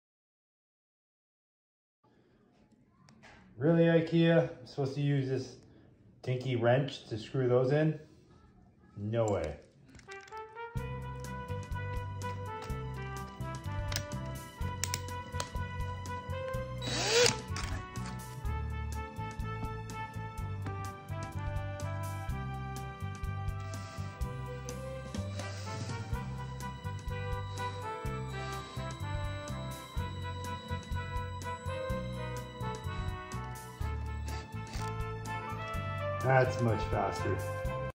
IKEA assembly hack
Hate those stupid tools IKEA gives you for their furniture? Here’s a quick hack to speed up your assembly!